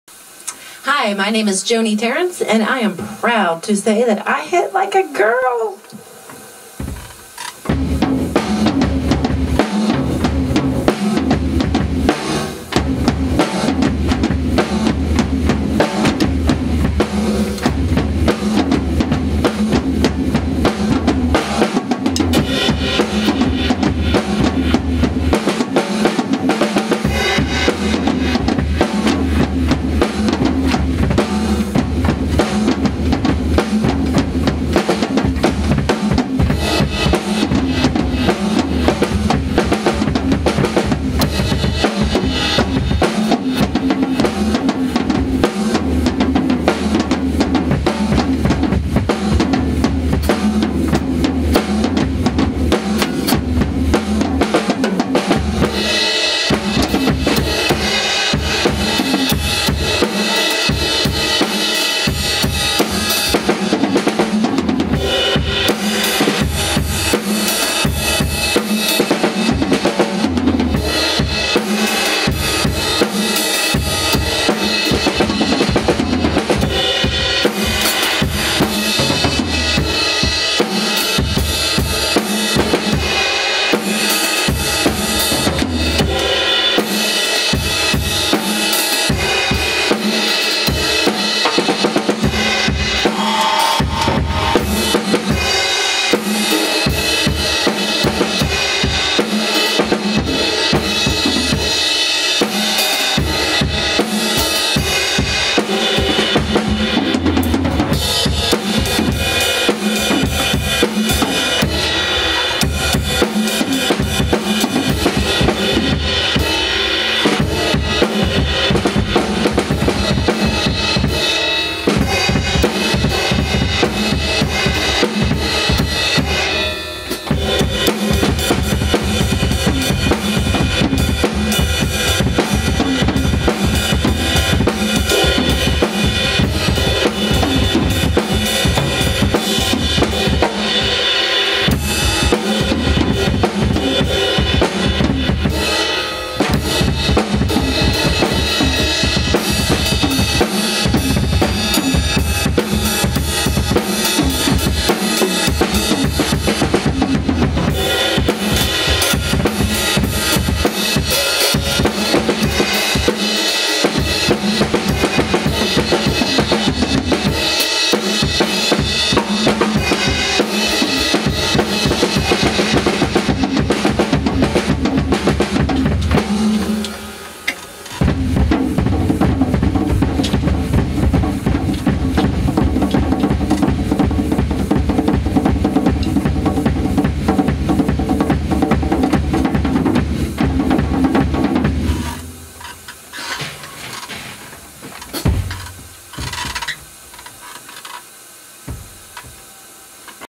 Joni 'BoneZ' Tarrance Hit Like a Girl Contest! 2016 Entry!!
Some raw footage of some solo stuff  by me video with Canon XA-10
be sure and blue for me this is my official Hit Like A girl Entry! and check out the other amazing female drummers out there, And Ladies... ENTER!!!

Hi everyone! My name is Joni 'BoneZ' Tarrance, I have played drums since age 10.   I came from a very musical family.  My mother is a drummer, and my father always played guitar, harmonica and sang, as a child I remember a bunch of their friends coming over and playing music till good knows when, lol, but I was able to sleep through it.  My mother had a kit in the living room as long as I can remember, in 6th grade I took up band in middle school choir one year, I have mostly (99.9%) taught myself by watching others and simply by listening to songs. I have been in several bands in Central VA, I am looking to further advance my skills, and hope top open a teaching studio someday soon. I want to target the female audience, because I only took only one year of band, and it's partially because I always felt intimidated/dominated by being the'only girl' and It is probably still that way.
   Drums are my passion, and I play daily.  I also play guitar, which I've learned soooooo much, with thanks to my husband, Chris, who is an amazing musician!  It's the greatest thing in the world having a studio in your basement and a partner to share your passion with! I am blessed.  Best luck to all who enter this amazing contest, I LOVE seeing all the other female drummers out there!